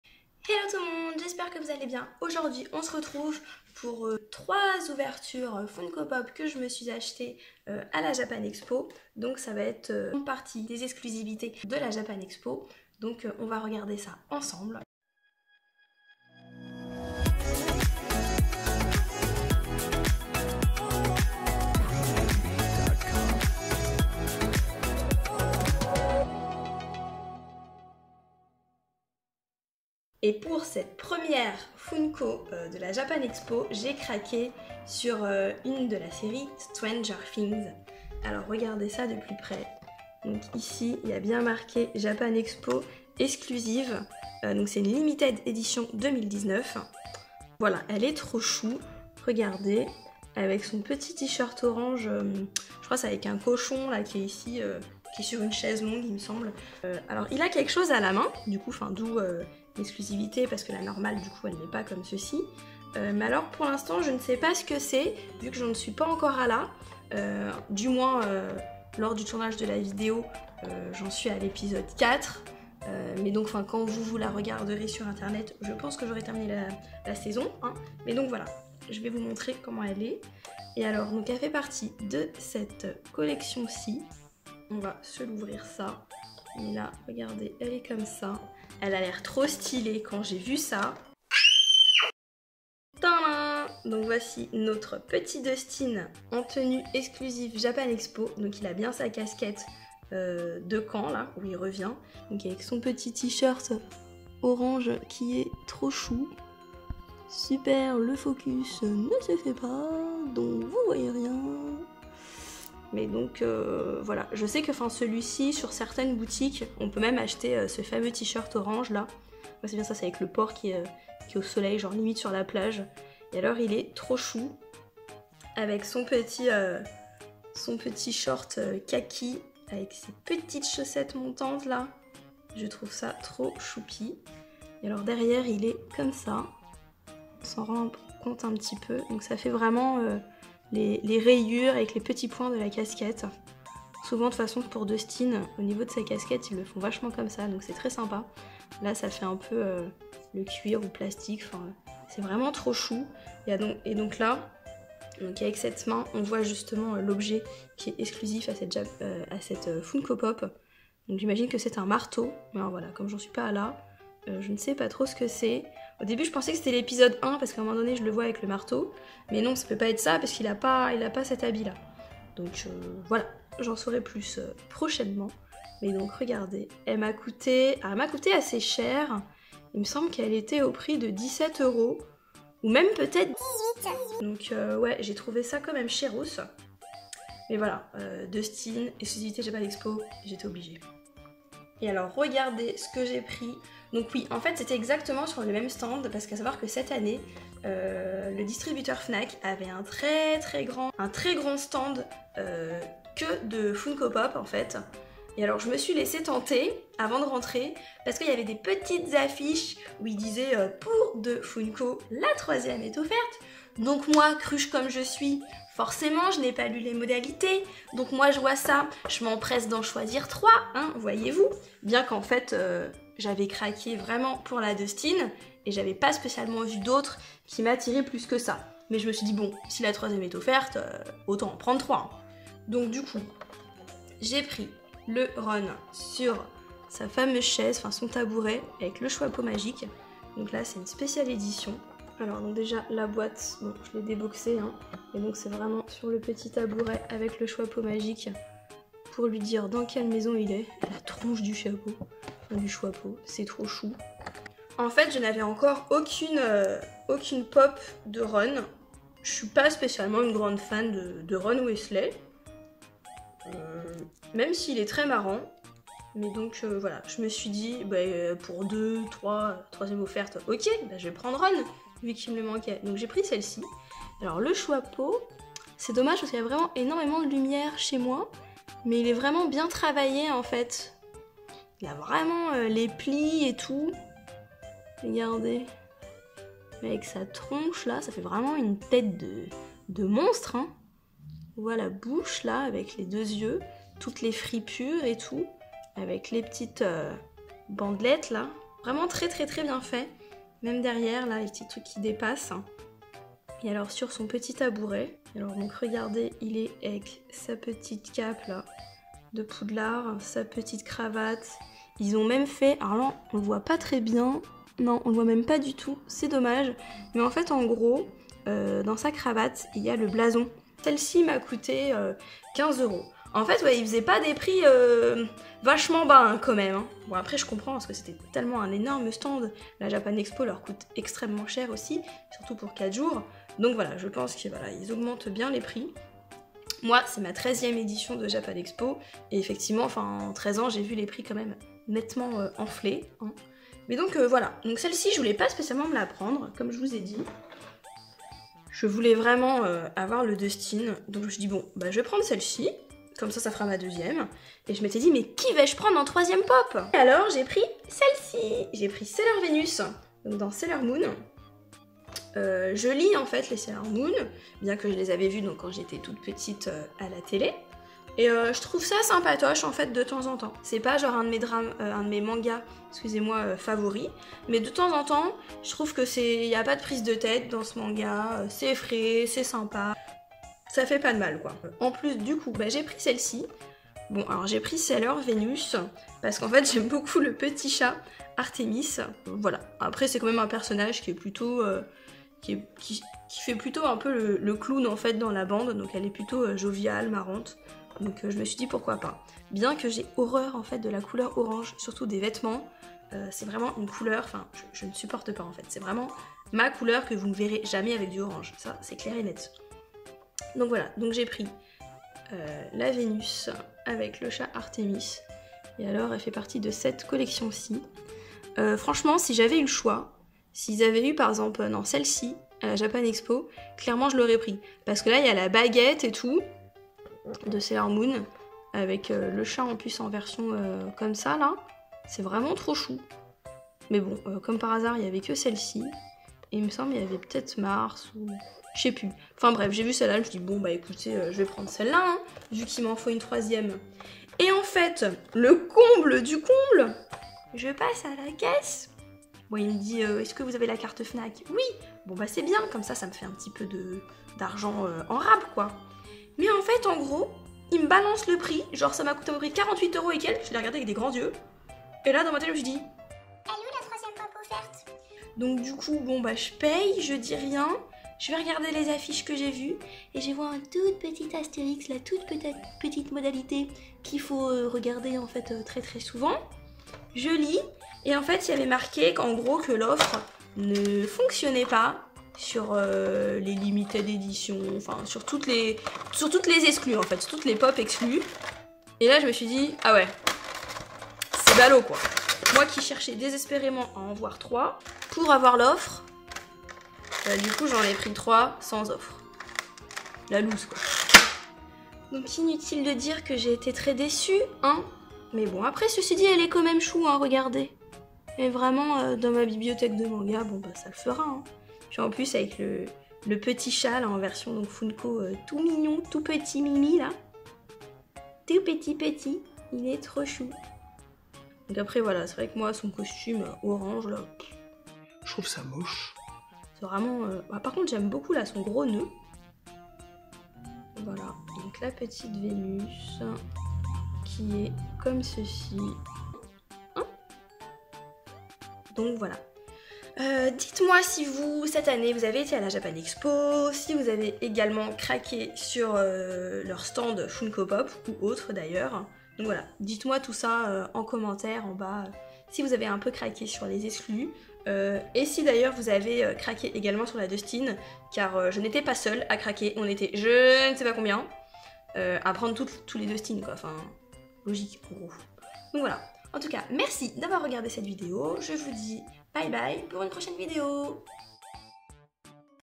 Funko pop Japan Expo exclusive! Avec Harry Potter!
Je vous montre dans cette vidéo, mes 3 Funko pop achetées à la Japan Expo 2019.

Une Funko Stranger things spéciale exclusive Japan Expo! Et j'ai nommé...
...Dustin!

Une limited edition d'Harry Potter : Ron avec le choipeaux magique!

Et une Funko normale, que vous pourrez trouver n'importe où : Sailor Venus, du manga Sailor Moon.

🎁 Sailor Venus - Artemis :